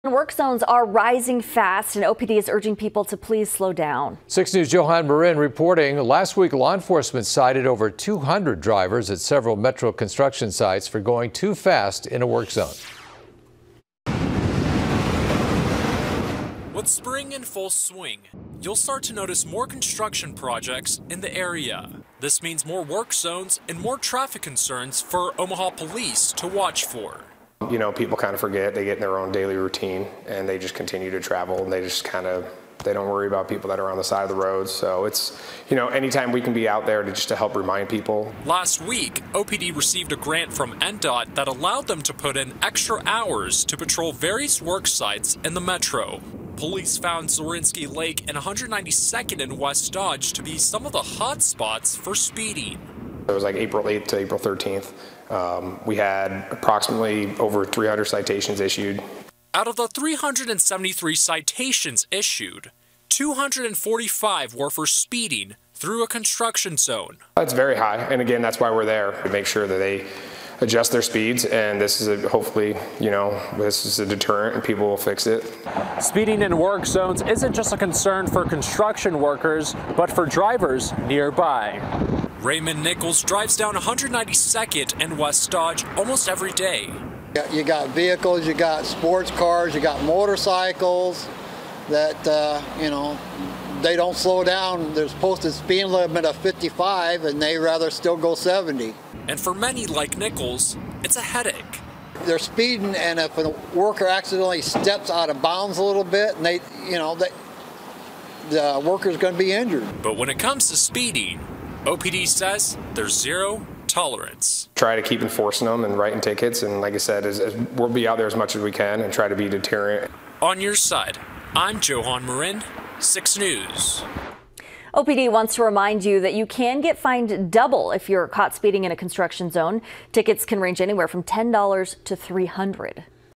Omaha Police crack down on speeding in work zones
Omaha Police are making an effort to crack down on an issue they say is getting worse: speeding through construction zones.